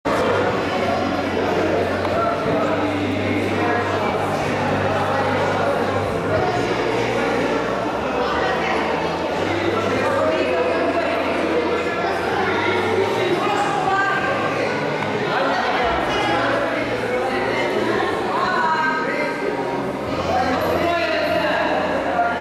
Ждем награждение.Турнир по дзюдо Дмитриевка. 19 сентября 2015.
Ждем награждение. 5-й Открытый Турнир по дзюдо, посвященный Дню празднования села. среди юношей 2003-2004, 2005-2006 г.р
с. Дмитриевка. 19 сентября 2015.